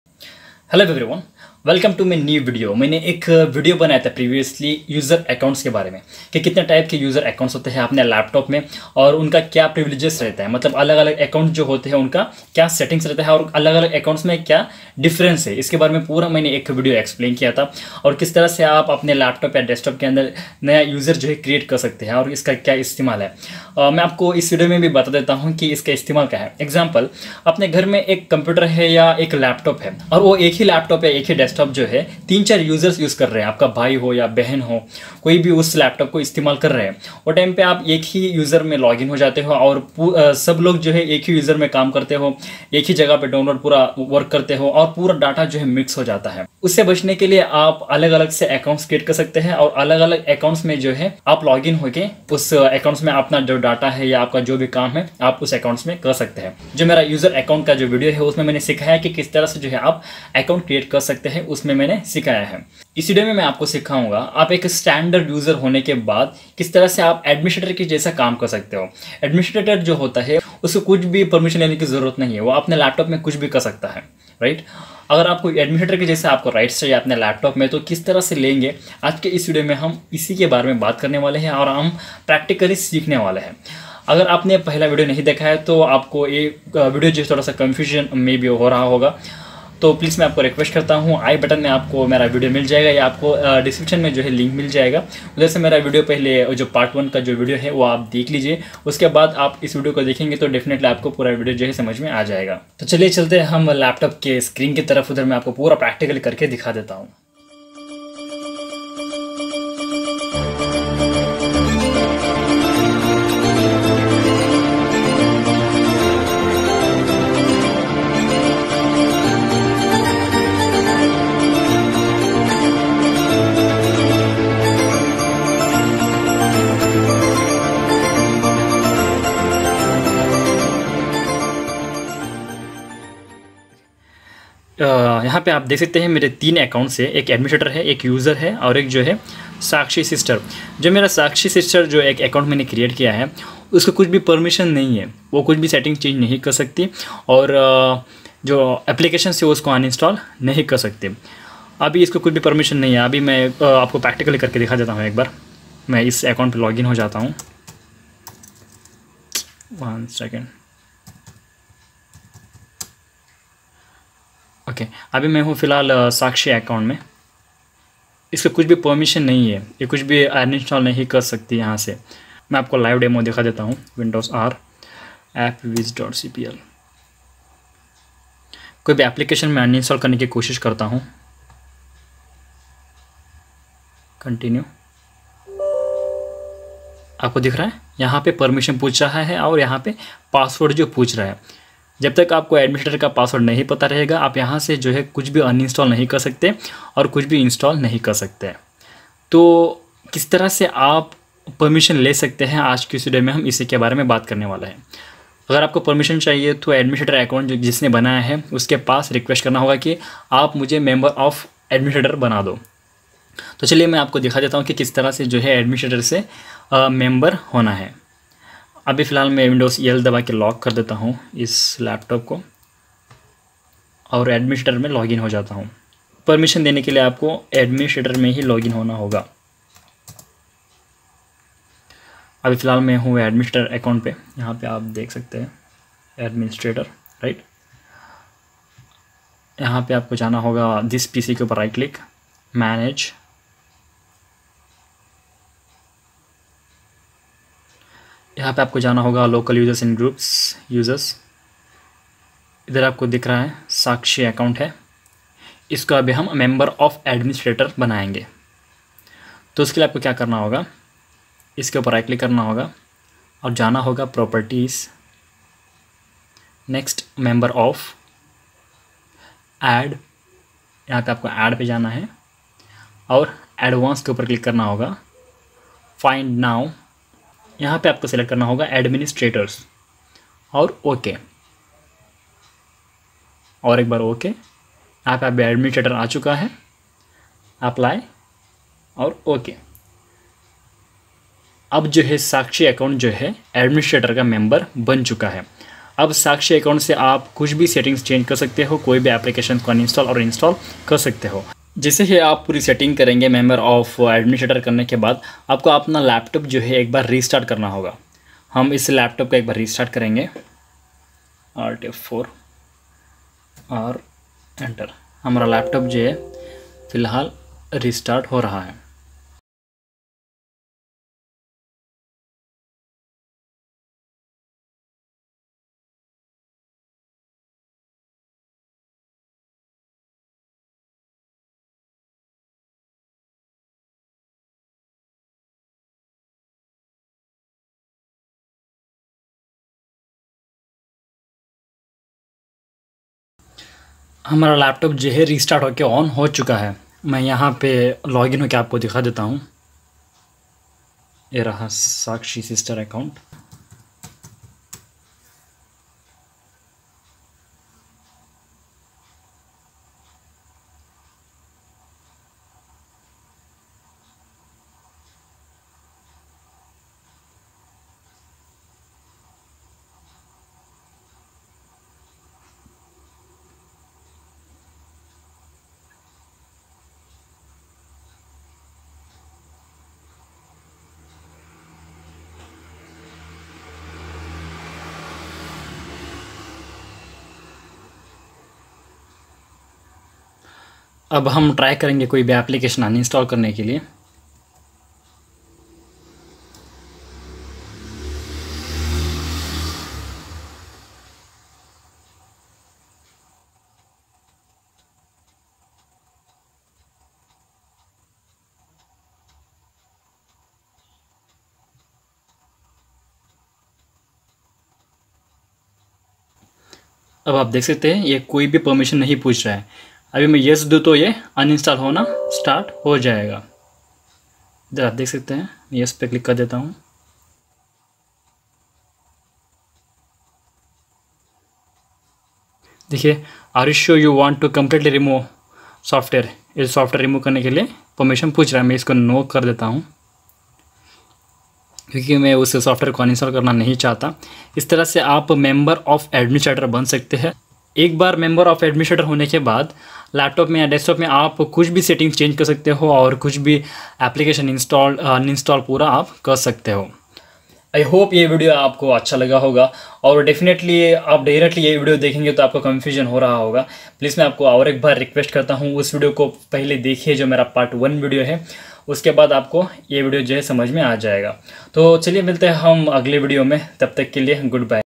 Become a member of administrator account || how to create multy account in windows 10 ||laptop basic
In this video, I have shown how you can become a member of administrator in windows 10 if you are a beginner please watch a full video.